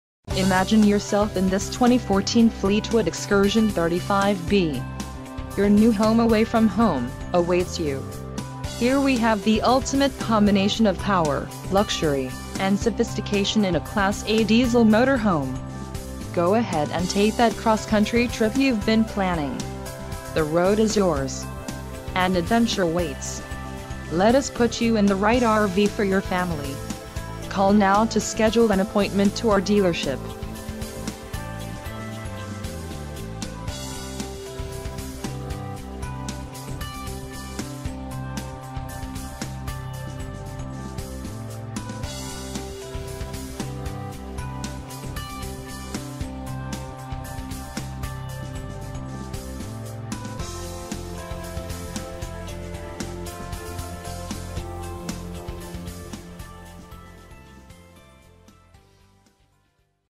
2014 Fleetwood Excursion - Niel's Motor Homes - North Hills
You\'ll appreciate the powerful 300 hp Freightliner chassis found in this Fleetwood Excursion. Our 35B model is a Class A Diesel motorhome with a central vacuum system and the satellite radio. You\'ll love the drop-down Queen bed and the slide out wardrobe along with the LED television. The spacious kitchen area includes a pair of barstools and a nearby luxurious L sofa. This 2014 model has gorgeous Tinted Pearl exterior and interior colors with Cinnabar cabinetry. , Tinted Pearl / Cinnabar\r\nFreigthliner Chassis 300hp\r\nBedroom Led Tv\r\nDriver / Passenger Center Table\r\nPower Passenger Seat Wht Power Footrest\r\nDrop Down Queen Bed\r\nAuto Generator Start\r\nSatellite Radio\r\nTinted Pearl\r\nCentral Vacuum System\r\nManual Roller Shades\r\nNeutral Loss Protection\r\nPower Driver\'s Seat